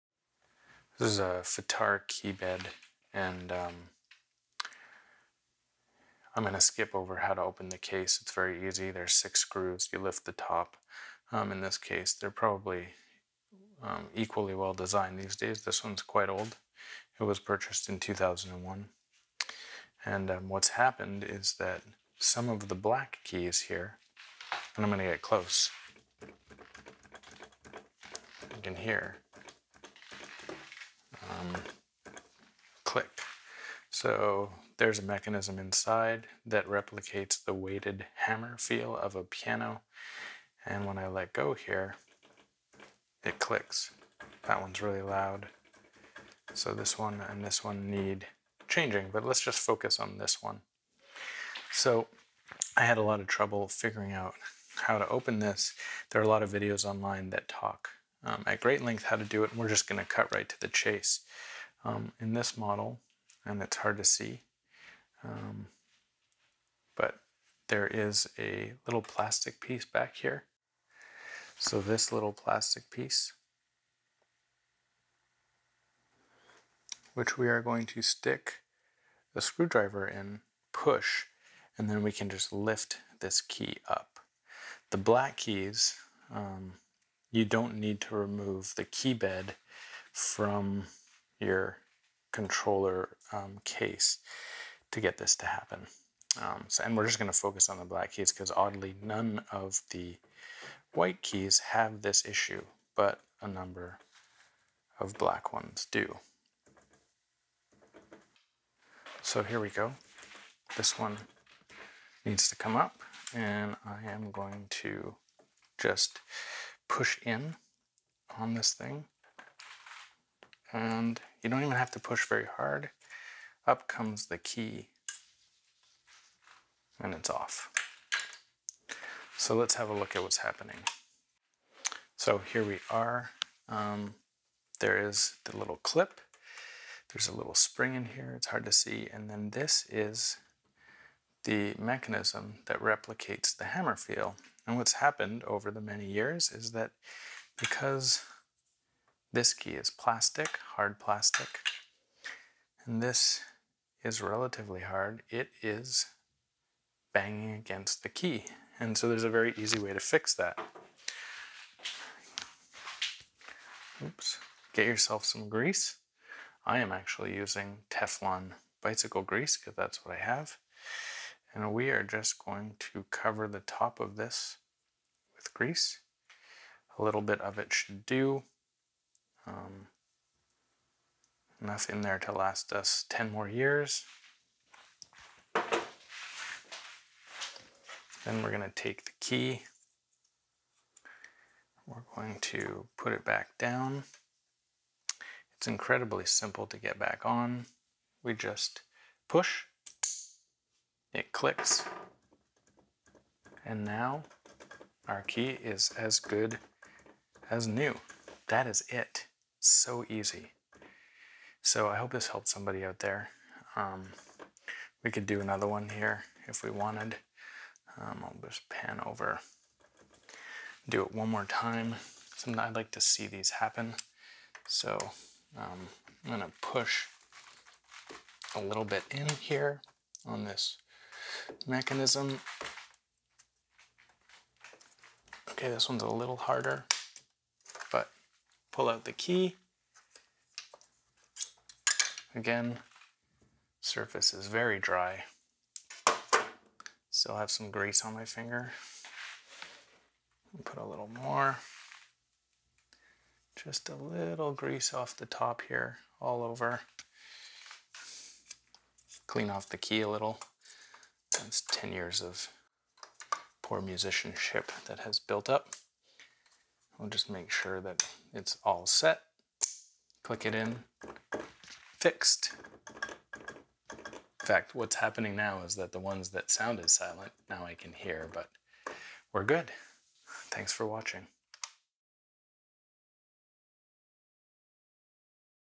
Fatar / StudioLogic "Clicky" key repair
A short demonstration on how to quickly remedy "clicky" keys on a Fatar/StudioLogic weighted keyboard.